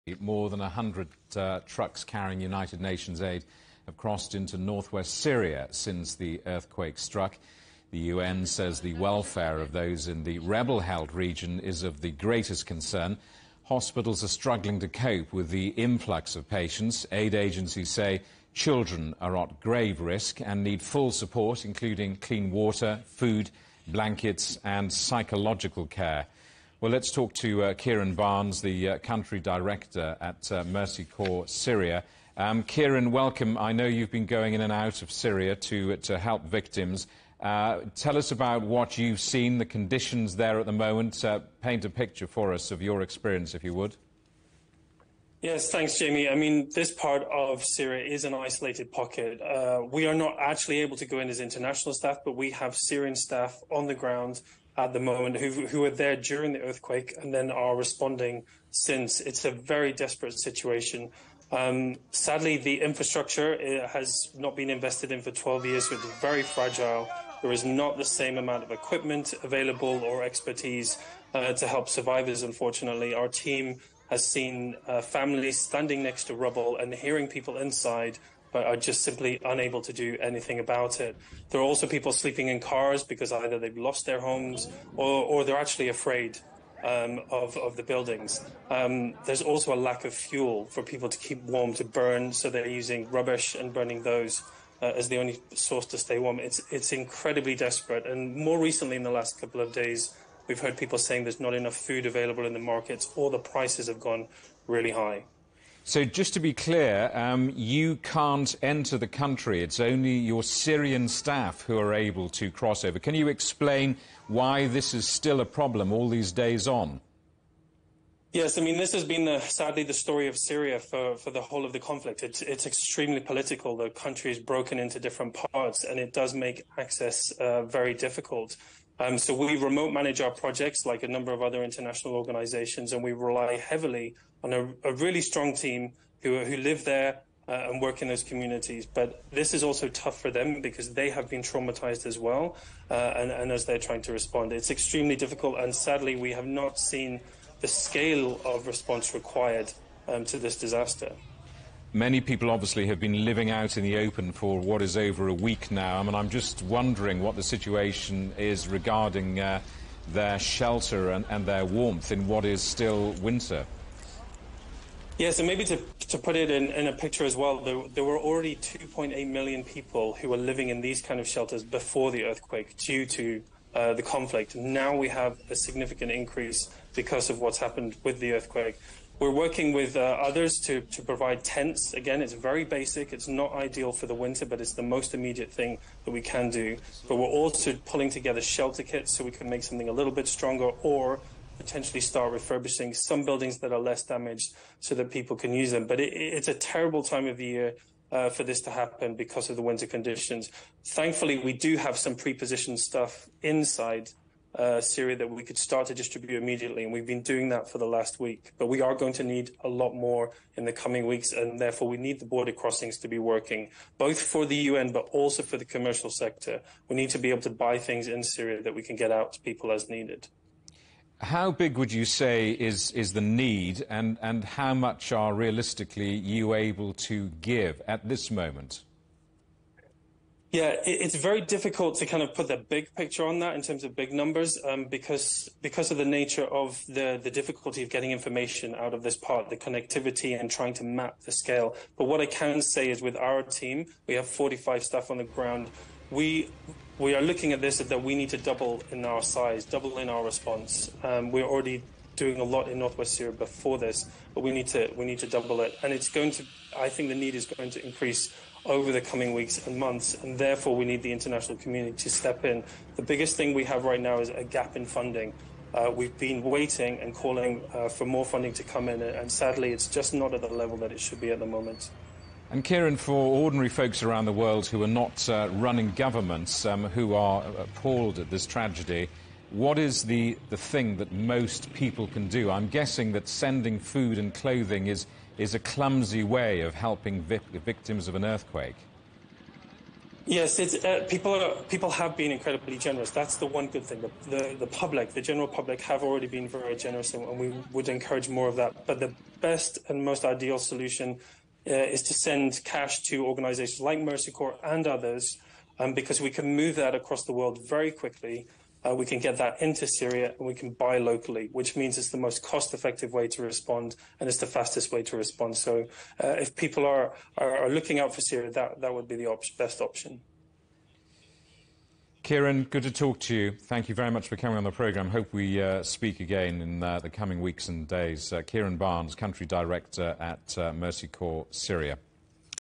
Türkiye Syria quake:"It's a very desperate situation"
CGTN Europe spoke to Kieren Barnes, Country Director, Mercy Corps Syria.